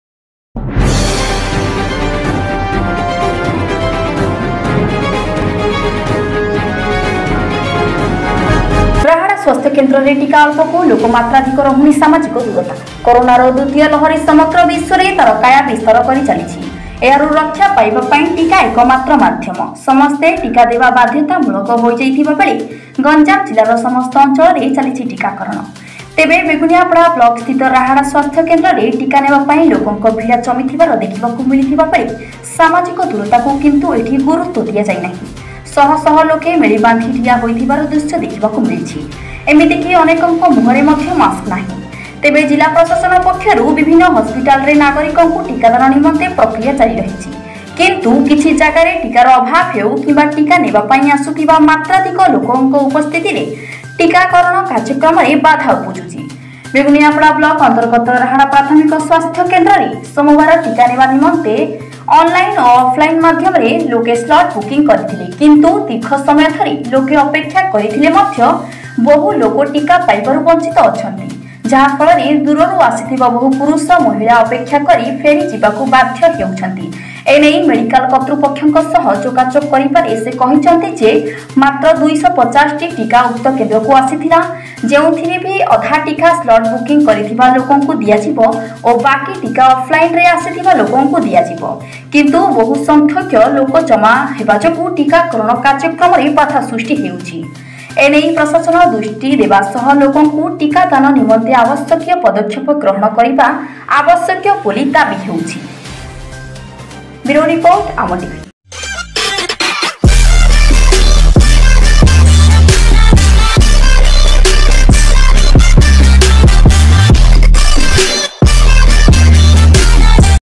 ରାହାଡା ସ୍ୱାସ୍ଥ୍ୟ କେନ୍ଦ୍ରରେ ଟୀକା ଅଳ୍ପ କୁ ଲୋକ ମାତ୍ରାଧିକ, ରହୁନି ସାମାଜିକ ଦୂରତା
କରୋନାର ଦ୍ୱତୀୟ ଲହରୀ ସମଗ୍ର ବିଶ୍ୱରେ ତାର କାୟା ବିସ୍ତାର କରିଚାଲିଛି |ଏହାରୁ ରକ୍ଷା ପାଇବା ପାଇଁ ଟିକା ଏକ ମାତ୍ର ମାଧ୍ୟମ । ସମସ୍ତେ ଟିକା ଦେବା ବାଧ୍ୟତା ମୁଳକ ହୋଇଯାଇଥିବା ବେଳେ ଗଂଜାମ ଜିଲ୍ଲା ର ସମସ୍ତ ଅଂଚଳରେ ଚାଲିଛି ଟିକାକରଣ  । ତେବେ ବେଗୁନିଆପଡା ବ୍ଲକ ସ୍ଥିତ ରାହାଡା ସ୍ୱାସ୍ଥ୍ୟ କେନ୍ଦ୍ରରେ ଟିକା ନେବା ପାଇଁ ଲୋକଙ୍କ ଭିଡ ଜମିଥିବାର ଦେଖିବାକୁ ମିଳିଥିବା ବେଳେ ସାମାଜିକ ଦୂରତା କୁ କିନ୍ତୁ ଏଠି ଗୁରୁତ୍ବ ଦିଆଯାଇ ନାହିଁ  । ଶହ ଶହ ଲୋକ ମେଳି ବାନ୍ଧି ଠିଆ ହୋଇଥିବା ର ଦୃଶ୍ଯ ଦେଖିବାକୁ ମିଳିଛି  । ଏମିତି କି ଅନେକଙ୍କ  ମୁହଁରେ ମଧ୍ୟ ମାସ୍କ ନାହିଁ ।ତେବେ  ଜିଲ୍ଲା ପ୍ରଶାସନ ପକ୍ଷରୁ ବିଭିନ୍ନ ହସ୍ପିଟାଲରେ ନାଗରିକଙ୍କୁ ଟୀକା ଦାନ ନିମନ୍ତେ ପ୍ରକ୍ରିୟା ଜାରୀ ରହିଛି | କିନ୍ତୁ କିଛି ଜାଗାରେ ଟୀକା ର ଅଭାବ ହେଉ କିମ୍ବା ଟୀକା ନେବା ପାଇଁ ଆସୁଥିବା ମାତ୍ରାଧିକ ଲୋକଙ୍କ ଉପସ୍ଥିତିରେ ଟୀକା କରଣ କାର୍ଯ୍ୟକ୍ରମରେ ବାଧା ଉପୁଜିଛି |  ବେଗୁନିଆପଡା ବ୍ଲକ ଅନ୍ତର୍ଗତ ରାହାଡା ପ୍ରାଥମିକ ସ୍ୱାସ୍ଥ୍ୟ କେନ୍ଦ୍ରରେ ସୋମବାର ଟୀକା ନେବା ନିମନ୍ତେ ଅନଲାଇନ ଓ ଅଫଲାଇନ ମାଧ୍ୟମରେ ଲୋକେ ସ୍ଲଟ ବୁକିଂ କରିଥିଲେ | କିନ୍ତୁ ଦୀର୍ଘ ସମୟ ଧରି ଲୋକେ ଅପେକ୍ଷା କରିଥିଲେ ମଧ୍ୟ ବହୁ ଲୋକ ଟୀକା ପାଇବାରୁ ବଞ୍ଚିତ ଅଛନ୍ତି | ଯାହା ଫଳରେ ଦୂରରୂ ଆସିଥିବା ବହୁ ପୁରୁଷ ମହିଳା ଅପେକ୍ଷା କରି ଫେରିଯିବାକୁ ବାଧ୍ୟ ହେଉଛନ୍ତି |ଏ ନେଇ ମେଡିକାଲ କର୍ତୃପକ୍ଷ ଙ୍କ ସହ ଯୋଗାଯୋଗ କରିବାରେ ସେ କହିଛନ୍ତି ଯେ ମାତ୍ର 250 ଟୀକା ଉକ୍ତ କେନ୍ଦ୍ରକୁ ଆସିଥିଲା, ଯେଉଁଥିରେ କି ଅଧା ଟୀକା ସ୍ଲଟ ବୁକିଂ କରିଥିବା ଲୋକଙ୍କୁ ଦିଆଯିବ ଓ ବାକି ଟୀକା ଅଫଲାଇନ ରେ ଆସିଥିବା ଲୋକଙ୍କୁ ଦିଆଯିବ | କିନ୍ତୁ ବହୁ ସଂଖ୍ୟକ ଲୋକ ଜମା ହେବା ଯୋଗୁଁ ଟୀକାକରଣ କାର୍ଯ୍ୟକ୍ରମରେ ବାଧା ସୃଷ୍ଟି ହେଉଛି | ଏ ନେଇ ପ୍ରଶାସନ ଦୃଷ୍ଟି ଦେବା ସହ ଲୋକଙ୍କୁ ଟୀକାଦାନ ନିମନ୍ତେ ଆବଶ୍ୟକୀୟ ପଦକ୍ଷେପ ଗ୍ରହଣ କରିବା ଆବଶ୍ୟକ ବୋଲି ଦାବୀ ହେଉଛି |